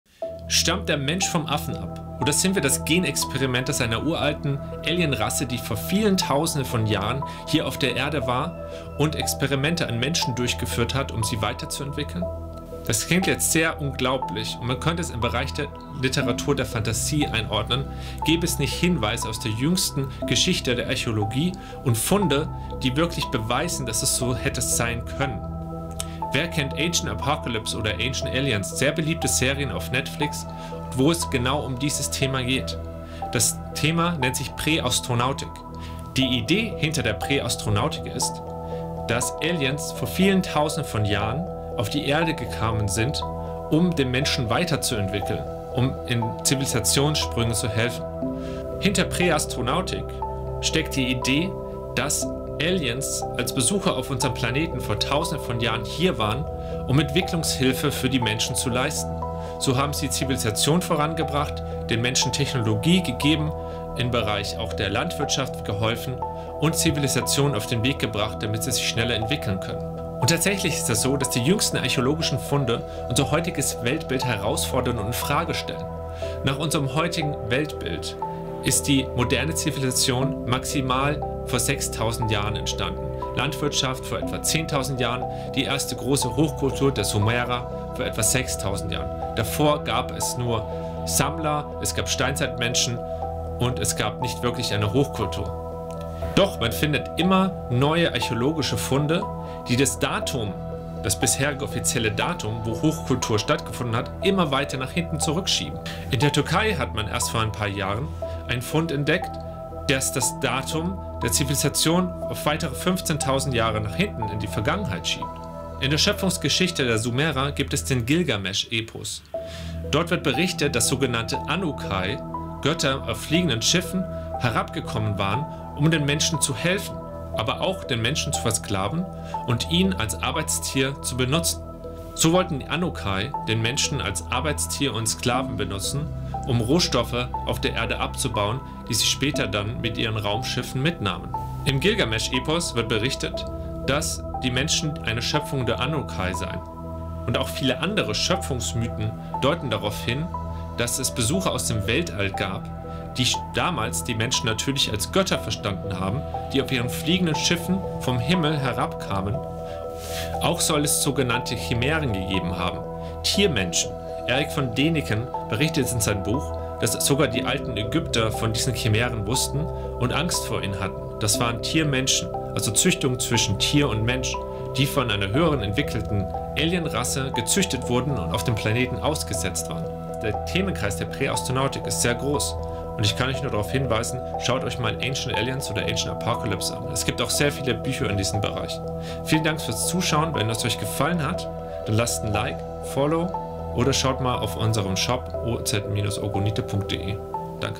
Ursprung des Menschen - Von Aliens gezüchtet?
In diesem Video erforschen wir die faszinierende Theorie der Prä-Astronautik. Stammen wir von Affen ab oder sind wir das Ergebnis eines außerirdischen Gen-Experiments? Wir beleuchten archäologische Funde und alte Schöpfungsmythen, ziehen Parallelen zu Serien wie "Ancient Aliens" und betrachten Erich von Dänikens Theorien. Ob Evolution oder Prä-Astronautik, die Frage nach dem Ursprung der Menschheit bleibt spannend und offen. Treten Sie ein in diese spannende Diskussion und erweitern Sie Ihr Wissen und Verständnis mit uns. Verpassen Sie nicht diesen aufschlussreichen Einblick in die menschliche Geschichte und Zukunft!